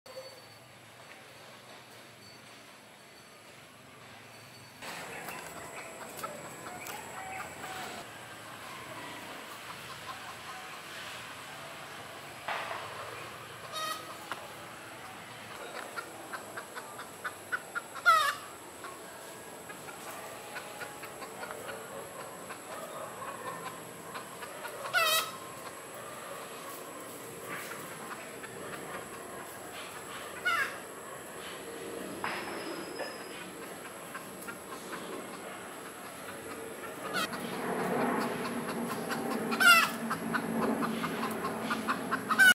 I follow this chicken like a predator
Is unusual for me to see a chicken on this part of forest so got curious.